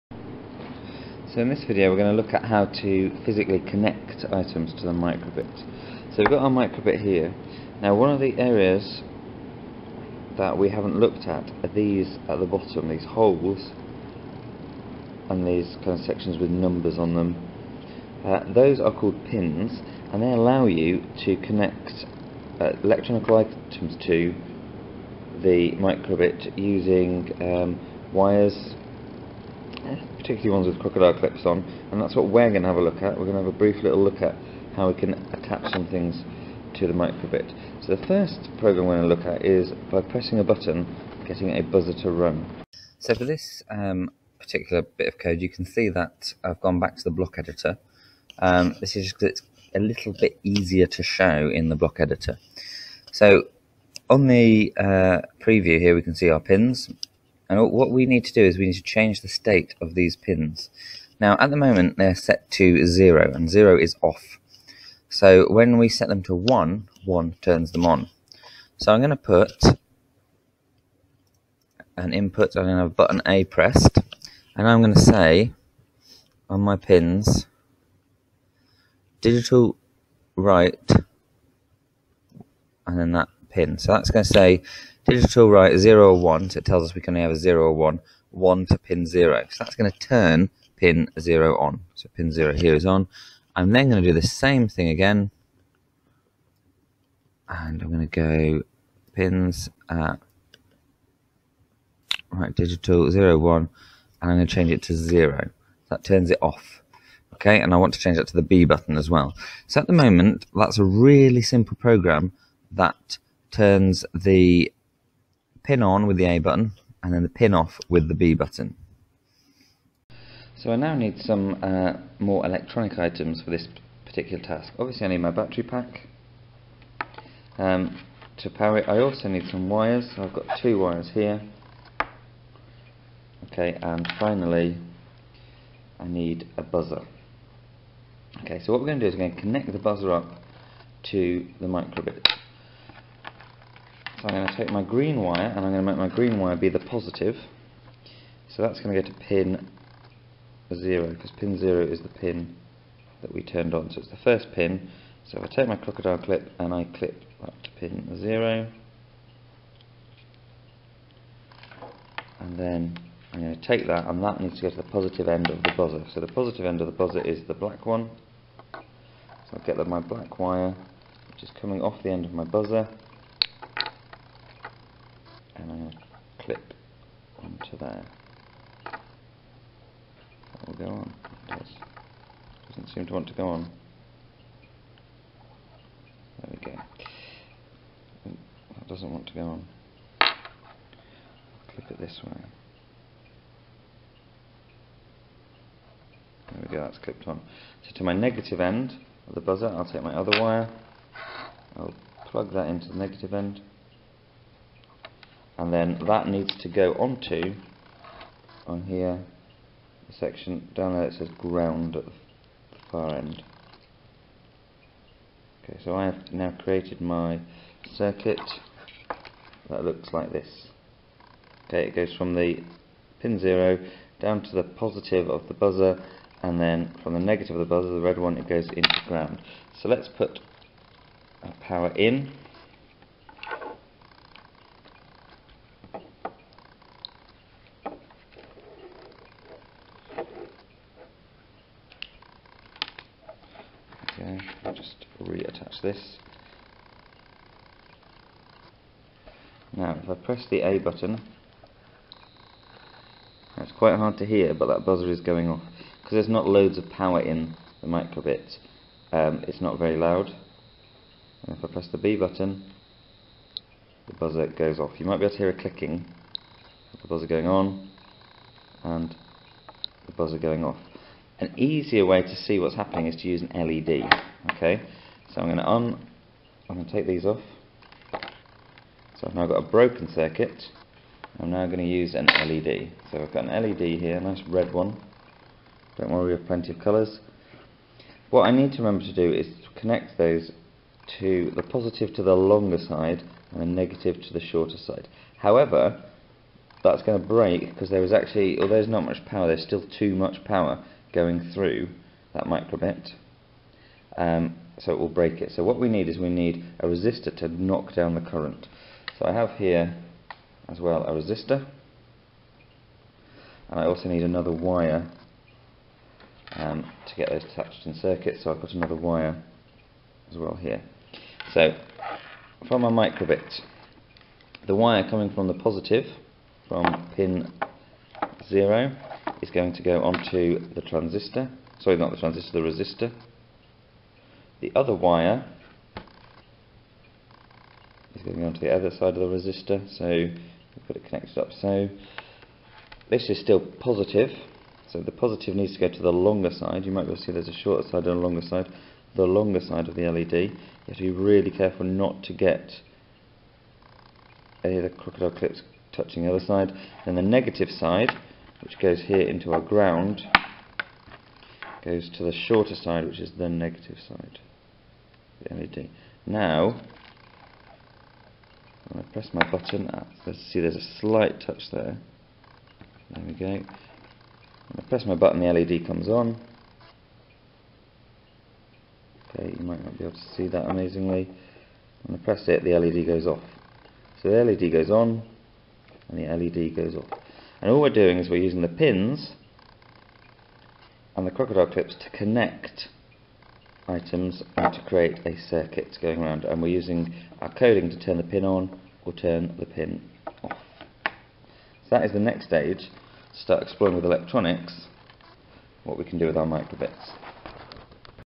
9 using pins to add electronic devices
How to add wires and simple electronic devices to a BBC micro:bit